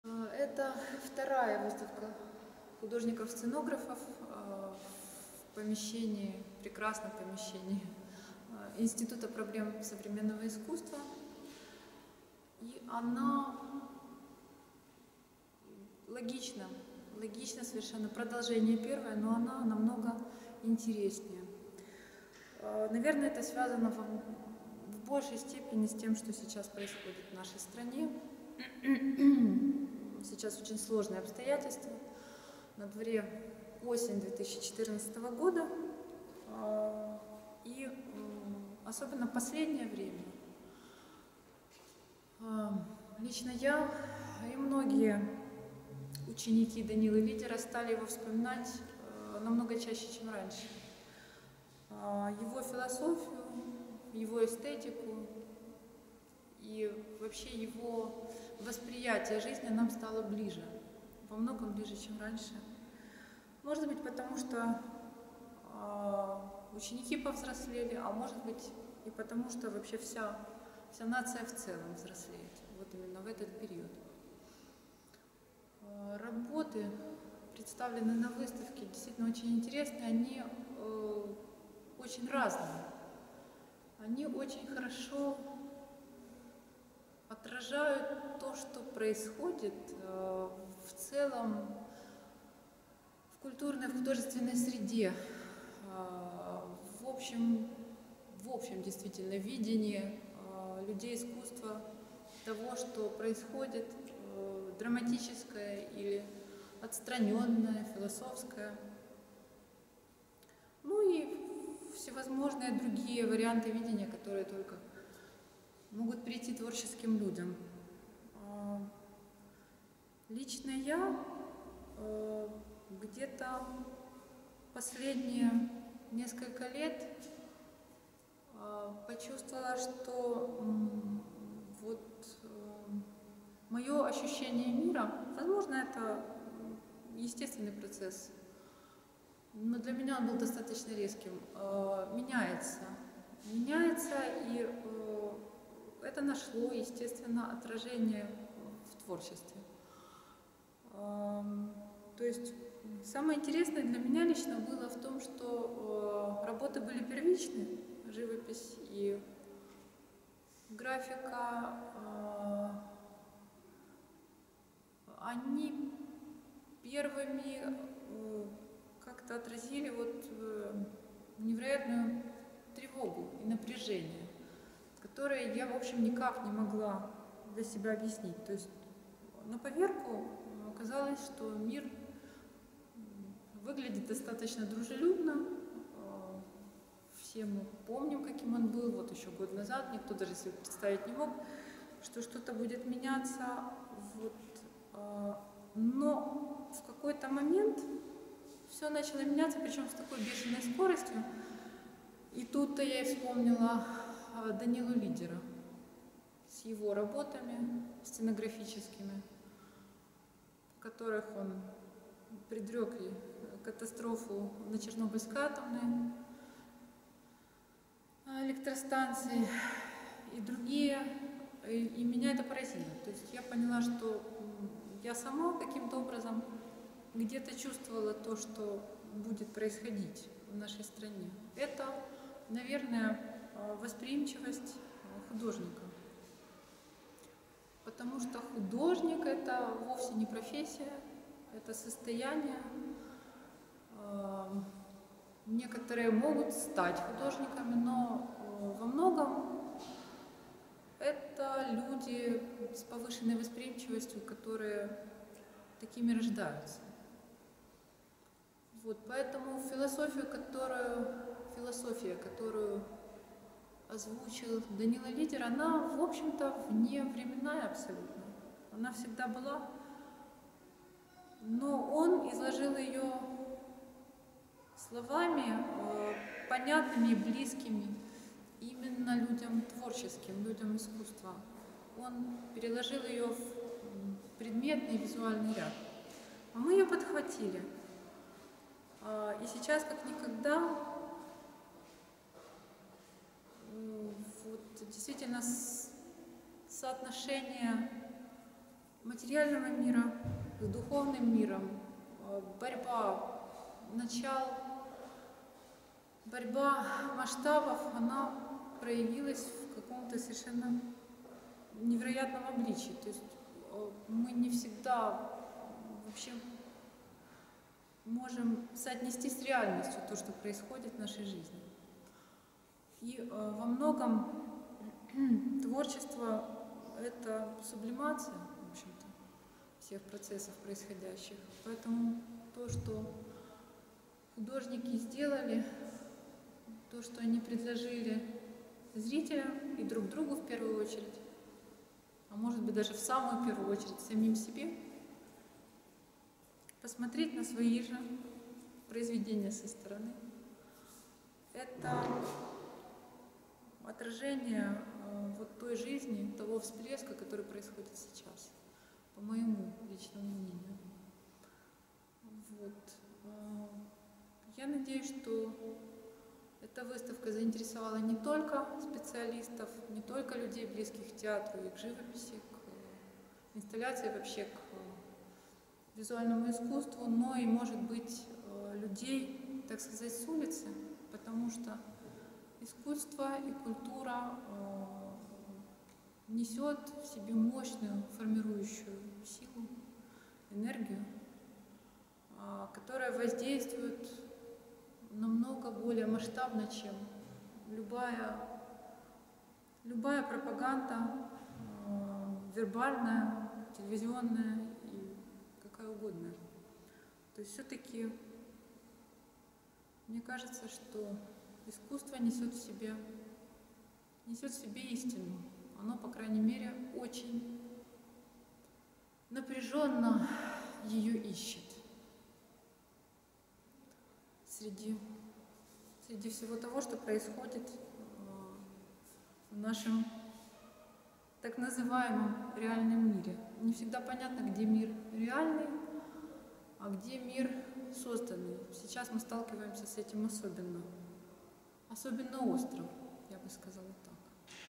Ганна Духовична – учасник виставки «Борітеся – поборете!».ІПСМ НАМ України. 28.08 — 14.09.2014
Національна академія мистецтв України
ІНСТИТУТ ПРОБЛЕМ СУЧАСНОГО МИСТЕЦТВА
ВСЕУКРАЇНСЬКА ВИСТАВКА СЦЕНОГРАФІВ
«БОРІТЕСЯ — ПОБОРЕТЕ!»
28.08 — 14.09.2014
Куратори: Олександр Клековкін, Сергій Васильєв, координатор: Зінаїда Шевченко

Виставку «Борітеся – поборете!»
 представляє куратор Сергій Васильєв: